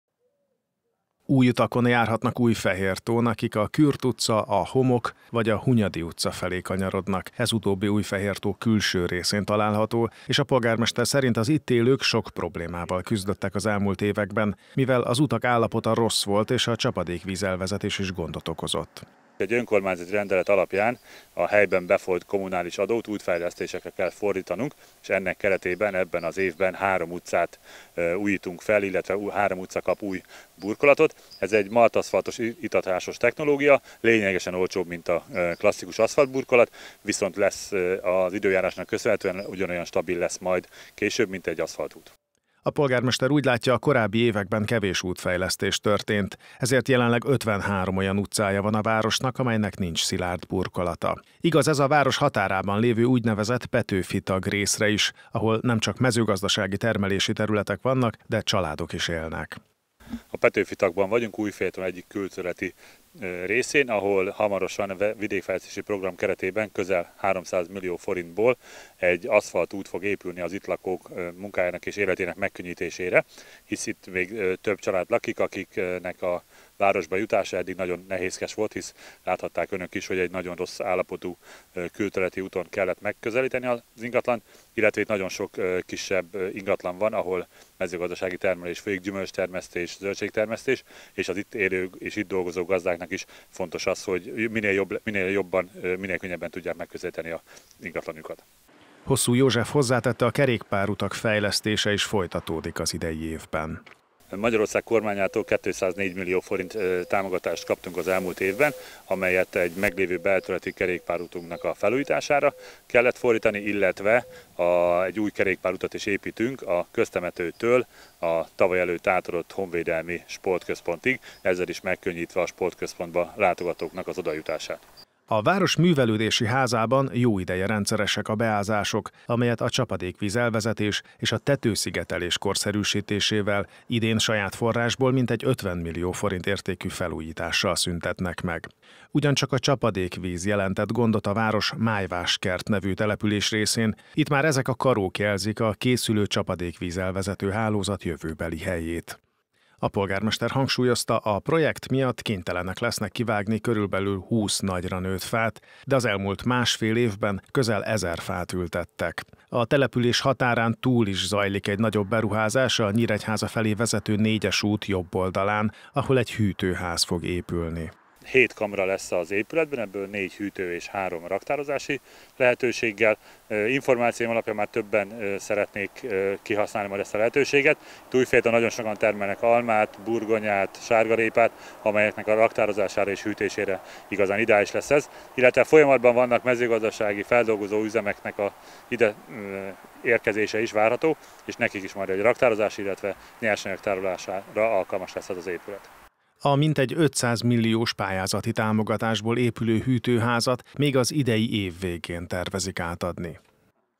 Milliárdos fejlesztések Újfehértón
Jelentős közútfejlesztési program zajlik Újfehértón is. Mintegy 6 kilométeren újul meg az aszfalt a városban. Emellett elkezdődött a művelődési ház korszerűsítése és a csapadékvíz-elvezető rendszer bővítése is a településen. Összességében mintegy 1,3 milliárd forintos fejlesztés fejeződhet be az év végére Újfehértón.